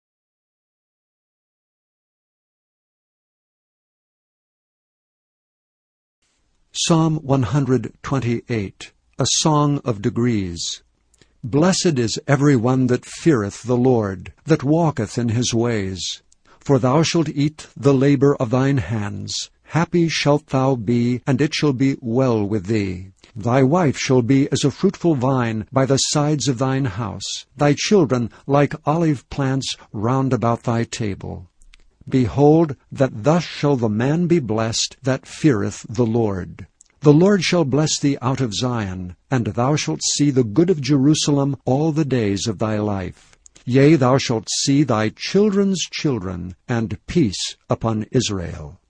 Psalm 128
Psalms Chapter 128. Text and Audio is from the King James Version of the Holy Bible.